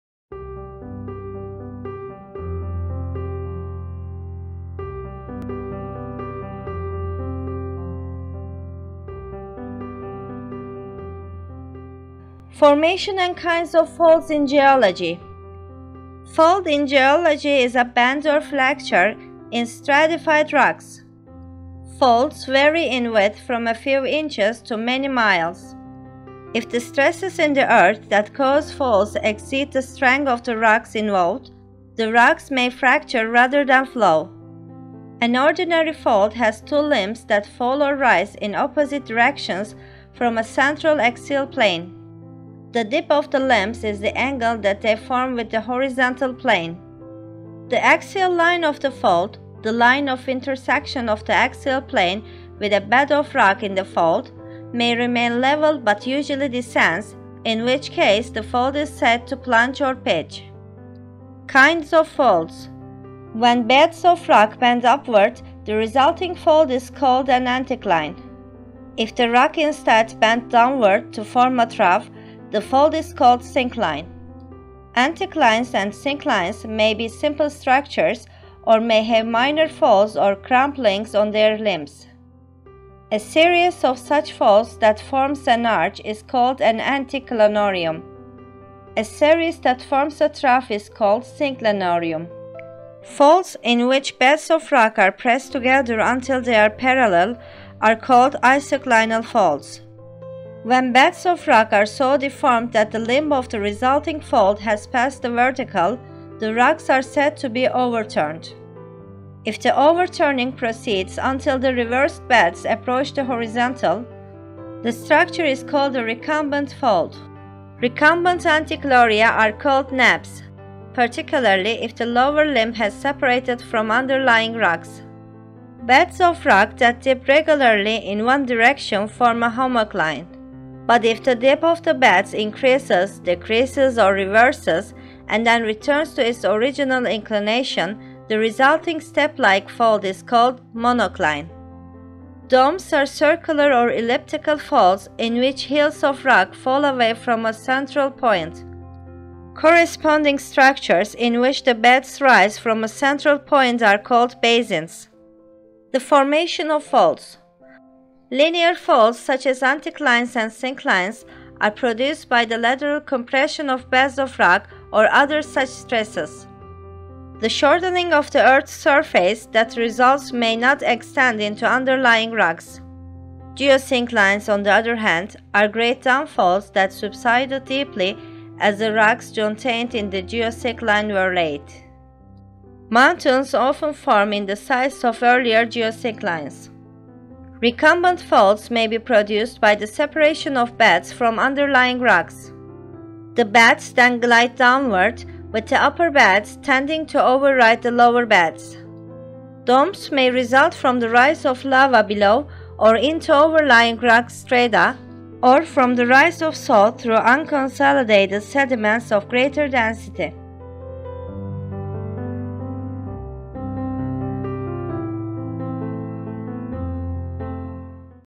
Formation and Kinds Of Folds (Geology) - What is fold? How do forms form?
What is fold? How do forms form? What are the kinds of folds? Information about the formation of folds.
Fold; in geology, a bend, or flexure, in stratified rocks. Folds vary in width from a few inches to many miles. If the stresses in the earth that cause folds exceed the strength of the rocks involved, the rocks may fracture rather than flow.

An ordinary fold has two limbs that fall or rise in opposite directions from a central axial plane. The dip of the limbs is the angle that they form with the horizontal plane. The axial line of the fold—the line of intersection of the axial plane with a bed of rock in the fold—may remain level but usually descends, in which case the fold is said to "plunge" or "pitch."

Kinds of Folds. When beds of rock bend upward, the resulting fold is called an anticline. If the rocks instead bend downward to form a trough, the fold is called a syncline. Anticlines and synclines may be simple structures or may have minor folds or crumplings on their limbs. A series of such folds that forms an arch is called an anticlinorium; a series that forms a trough is called a synclinorium.

Folds in which beds of rock are pressed together until they are parallel are called isoclinal folds. When beds of rock are so deformed that the limb of the resulting fold has passed the vertical, the rocks are said to be overturned. If the overturning proceeds until the reversed beds approach the horizontal, the structure is called a recumbent fold. Recumbent anticlinoria are called nappes, particularly if the lower limb has separated from underlying rocks.

Beds of rock that dip regularly in one direction form a homocline. But if the dip of the beds increases, decreases, or reverses, and then returns to its original inclination, the resulting steplike fold is called a monocline.

Domes are circular or elliptical folds in which heels of rock fall away from a central point. Corresponding structures in which the beds rise from a central point are called basins.

The Formation of Folds. Linear folds such as anticlines and synclines are produced by the lateral compression of beds of rock or other such stresses. The shortening of the earth's surface that results may not extend into underlying rocks. Geosynclines, on the other hand, are great down-folds that subsided deeply as the rocks jontained in the geosyncline were laid. Mountains often form in the sites of earlier geosynclines.

Recumbent folds may be produced by the separation of beds from underlying rocks. The beds then glide downward, with the upper beds tending to override the lower beds. Domes may result from the rise of lava below or into overlying rock strata, or from the rise of salt through unconsolidated sediments of greater density.